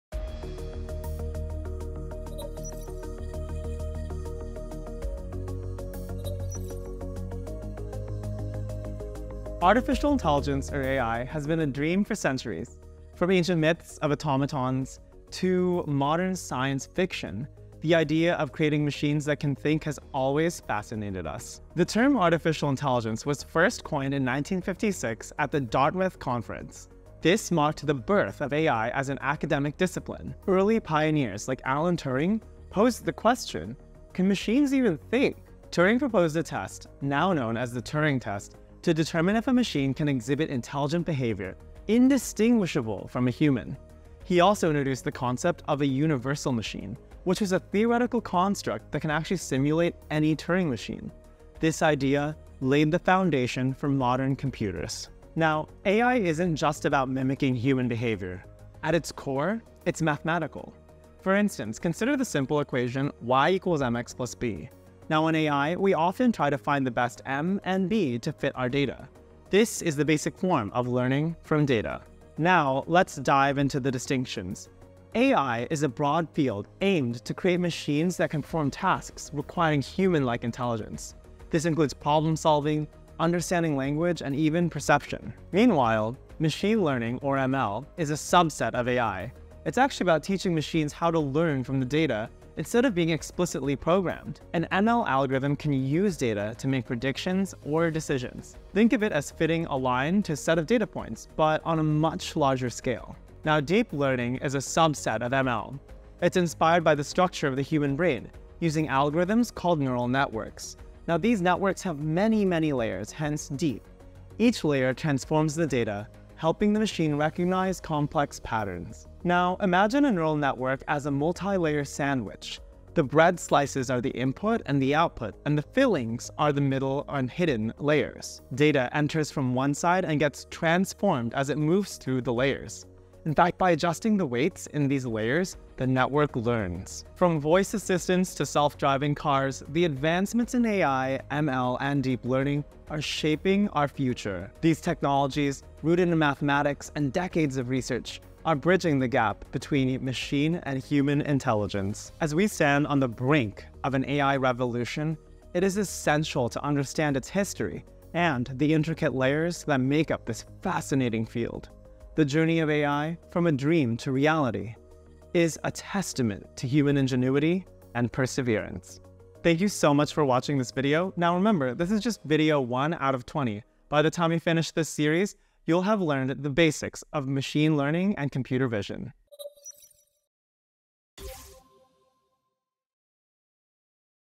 A Brief History of AI: AI and Machine Learning Course for Beginners
This is video 1 out of 20 in our AI and Machine Learning Course for Beginners by Sean Batir: A Brief History of AI. It provides a concise overview of the history of AI and the distinctions between AI, ML, and Deep Learning, with mathematical references suitable for a high school graduate level audience.

Artificial Intelligence, or AI, has been a dream for centuries. From ancient myths of automatons to modern science fiction, the idea of creating machines that can think has always fascinated us.

00:00 Introduction
00:21 What is AI?
00:44 The Concept of a 'Universal Machine'
01:15 The Differences between AI and ML
01:43 What is Deep Learning?
02:17 How Do We Use AI?

Next videos will be released on January 19, 2024.